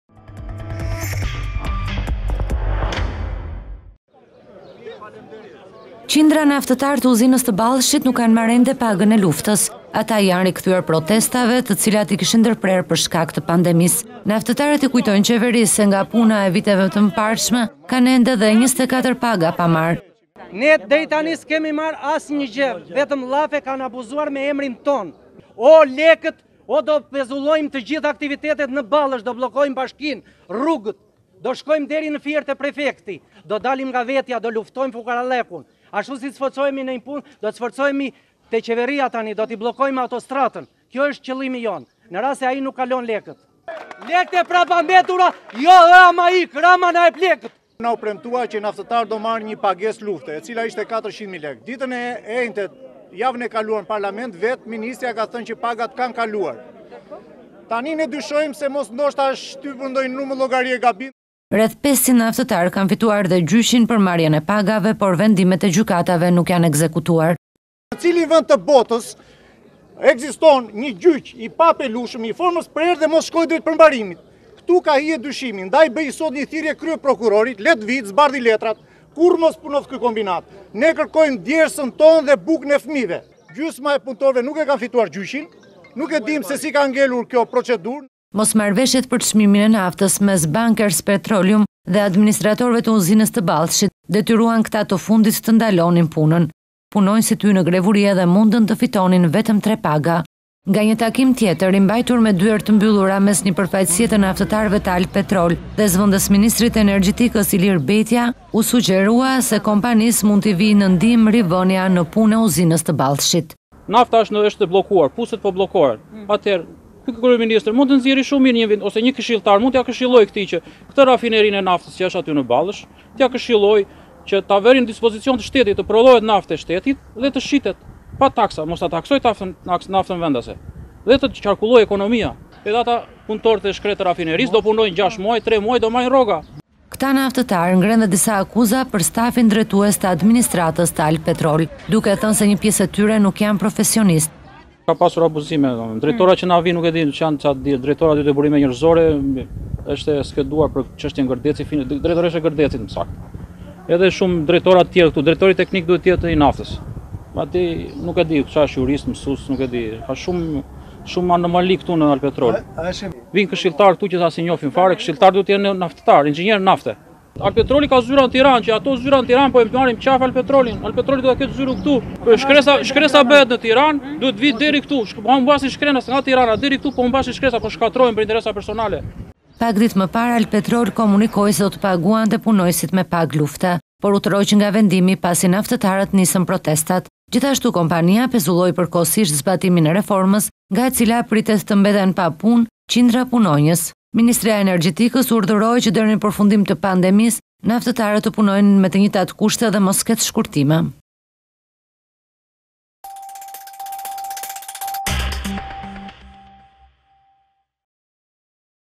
Naftetaret e Ballshit ne proteste: Duam pagen e luftes, punonjesit e albpetrolit sugjerojne zgjidhje
Qindra naftëtarë të uzinës së Ballshit nuk kanë marrë ende pagën e luftës. Ata i janë rikthyer protestave, të cilat i kishin ndërprerë për shkak të pandemisë. Naftëtarët i kujtojnë qeverisë se nga puna e viteve të mëparshme kanë ende 24 paga pa marrë.